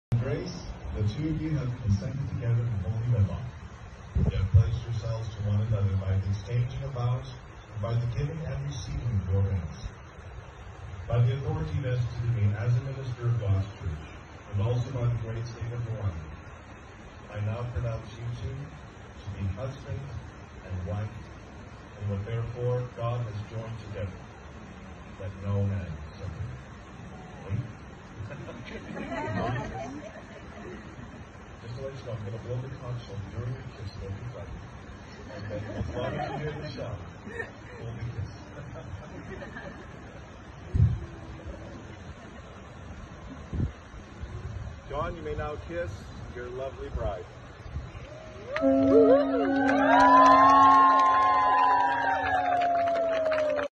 Wedding Jon & Grace - Pastor blowing the conch while groom kissing the bride - f38e33
Wedding Jon & Grace - Hawaii Pastor blowing the conch while groom kissing the bride